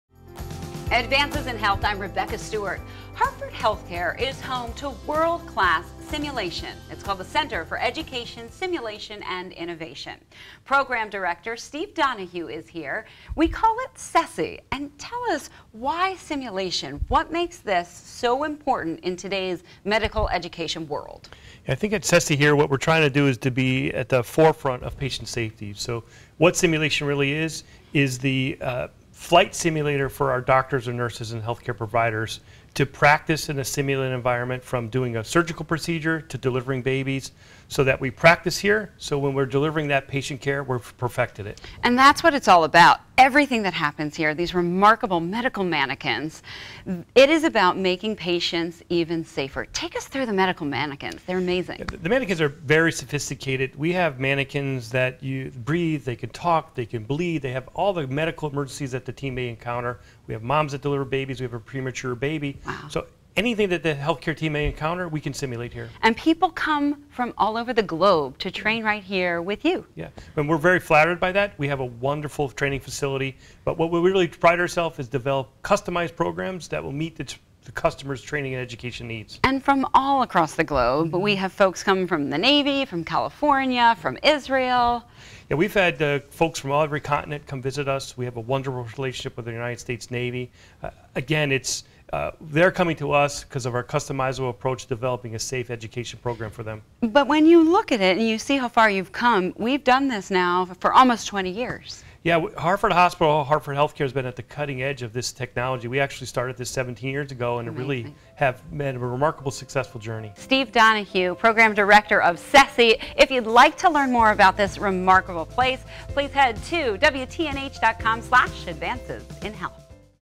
Advances in Health: The Center for Education, Simulation and Innovation
The Hartford HealthCare Center for Education, Simulation and Innovation: training tomorrow's clinicians today, and keep today's clinician in the know about the latest technology. Hartford HealthCare's Stephen Donahue explains.